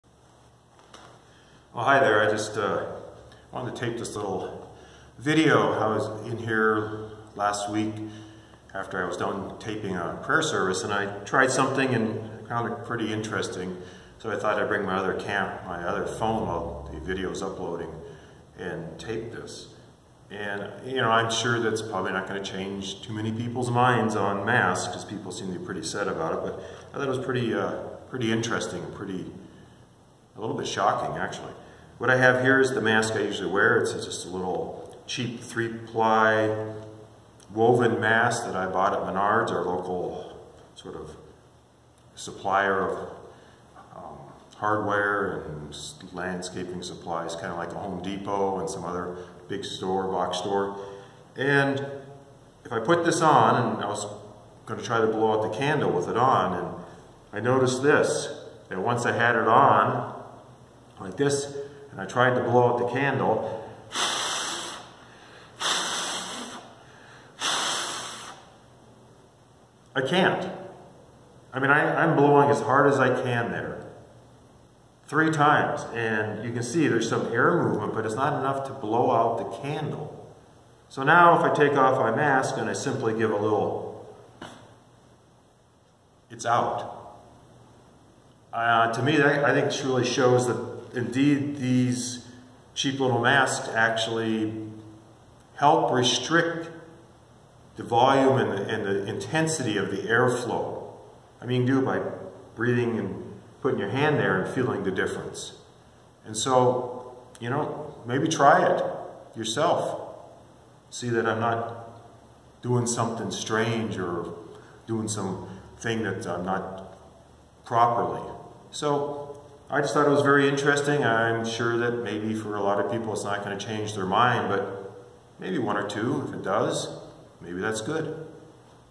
Masks do they work? Blowing out a candle.
Last week, when I tried to blow out the alter candles after videoing a prayer service I noticed that I could not do it with a cheap 3 layer woven mask on. I found this a pretty amazing example of how much these masks do impede airflow and the velocity of the air being expelled by a person.

I encourage you to try the same thing at home to see the difference. A simple puff of air is required to blow out the candle when I do not have the mask on, but I cannot blow hard enough to blow out the candle with the mask on.

While this is a nice easy and visual show of how masks work, I am sure this will not convince many people that are opposed to wearing a mask but if it does for a few that is good.